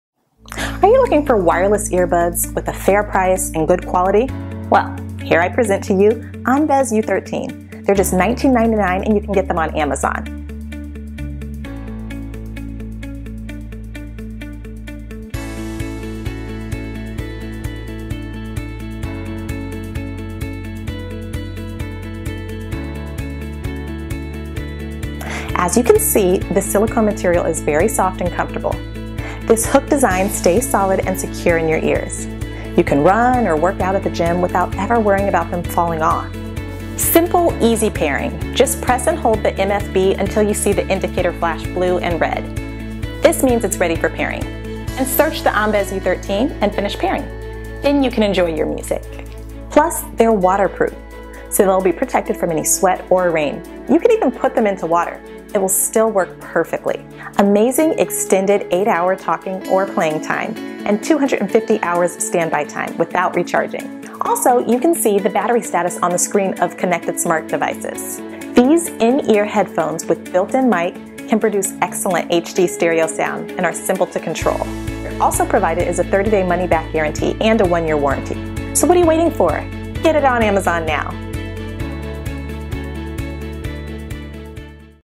ANBES U13 Wireless Bluetooth headphones review and tutorial
ANBES Bluetooth Headphones Wireless Earbuds, IPX7 Waterproof in-Ear Earphones Sports with Ear Hooks & Mic, HD Stereo Sound, Up to 8 Hours Playing Noise Canceling Headsets (Green Black)